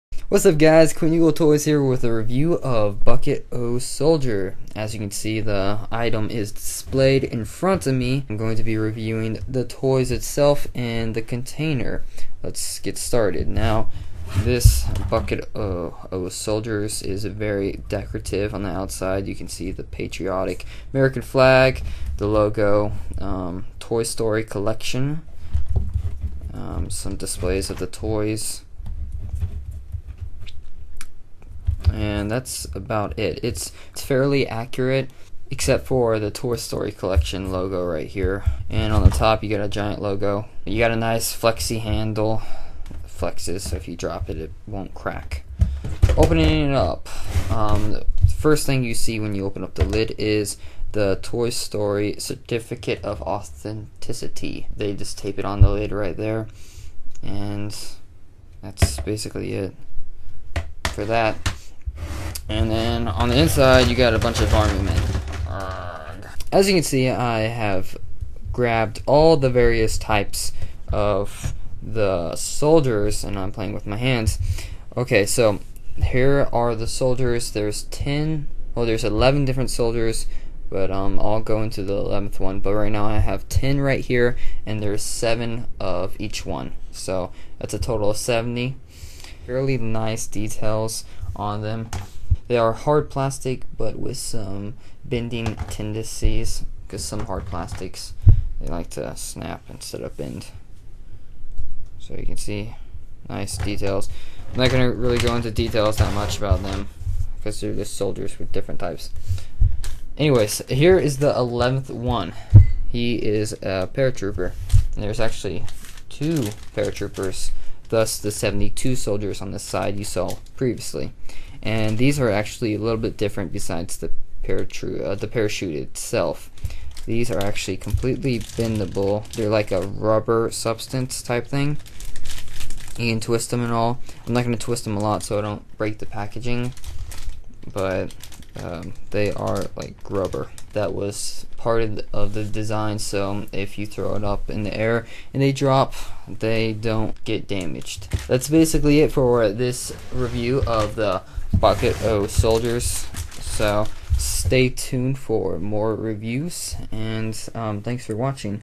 Bucket O Soldiers - Toy Story Collection
This is a video review of the Toy Story Collection Bucket O Soldiers. This costs around $10 in stores.